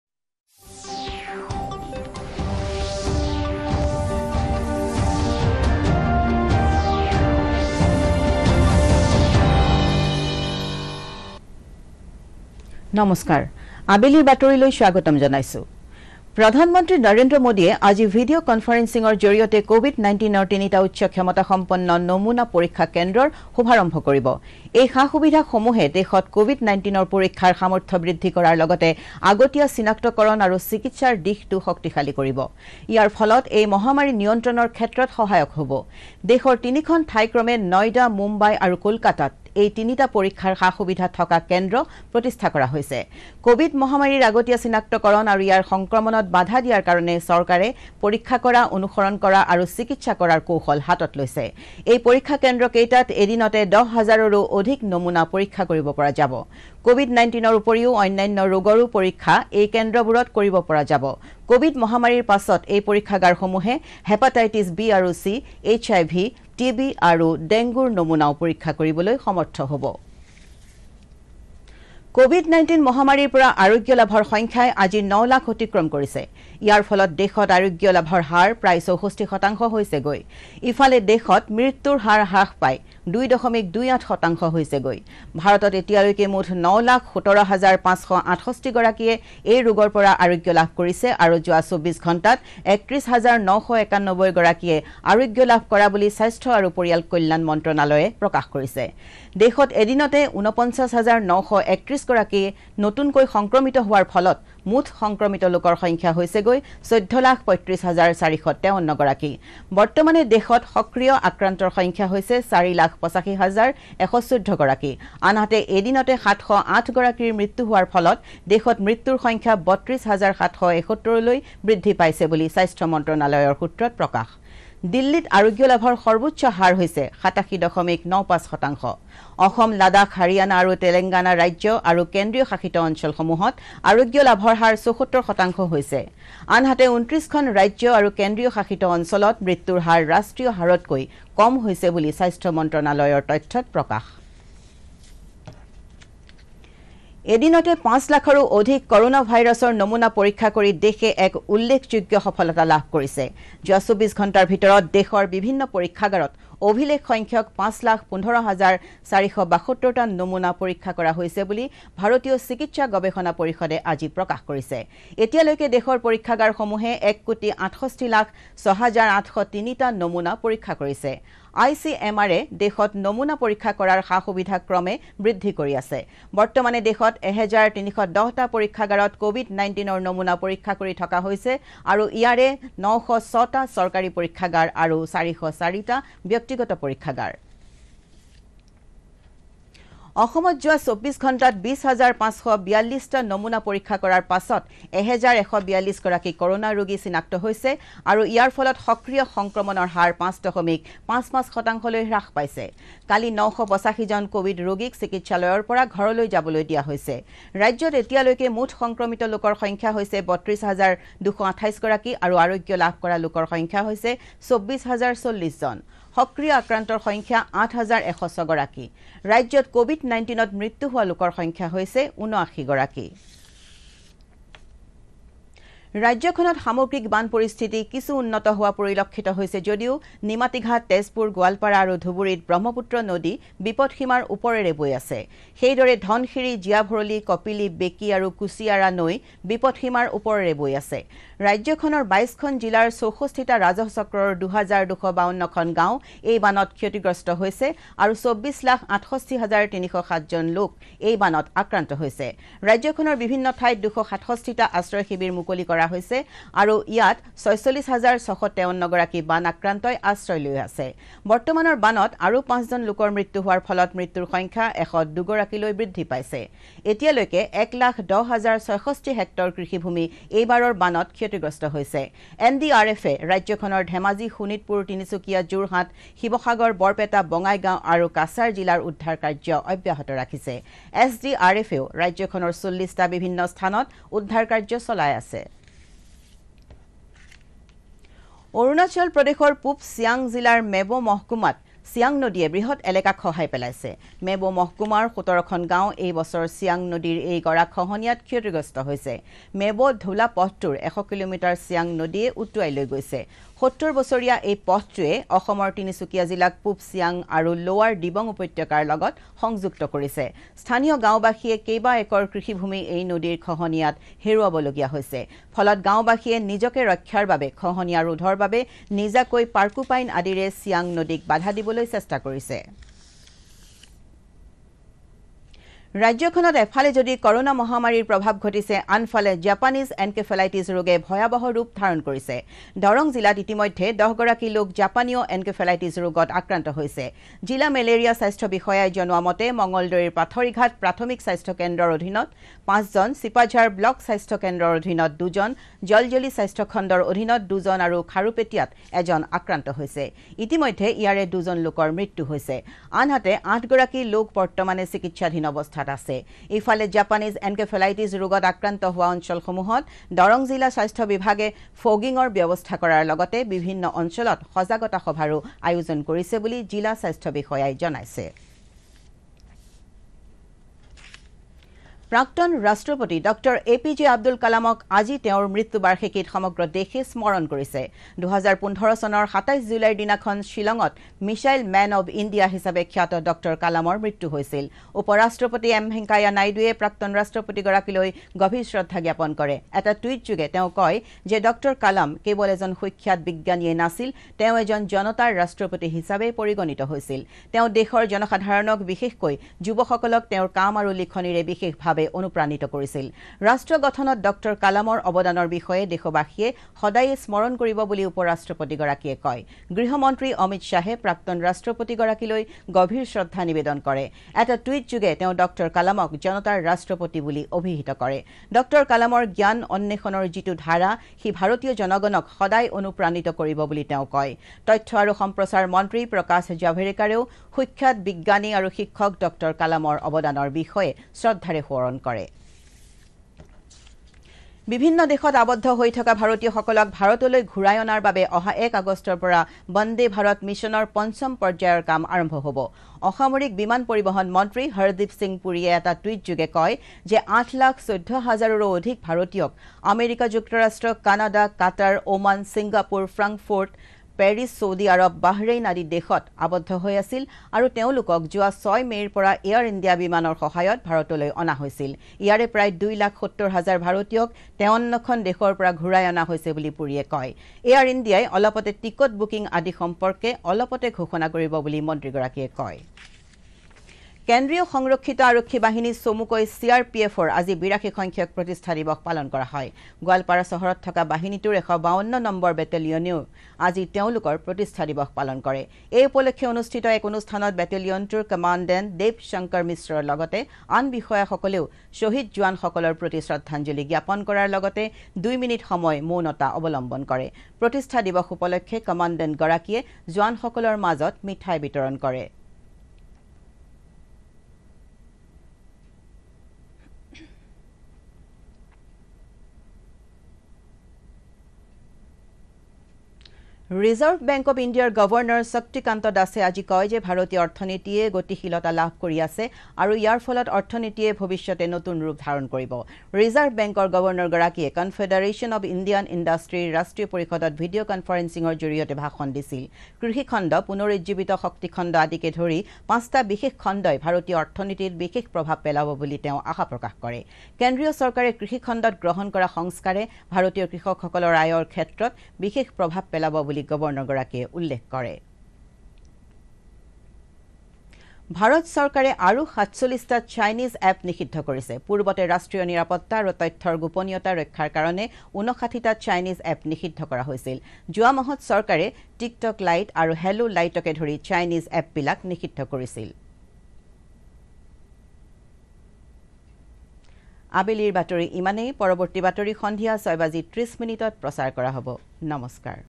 বাতৰি - Evening 4.00 PM Assamese News
এদিনতে ৫ লাখৰো অধিক কোৱিড-19ৰ নমুনা পৰীক্ষা কৰি দেশৰ এক উল্লেখযোগ্য সফলতা লাভ

মৃত্যু বাৰ্ষিকীত প্ৰাক্তন ৰাষ্ট্ৰপতি ড০ এ পি জে আব্দুল কালামক সমগ্ৰ দেশৰ সোঁৱৰণ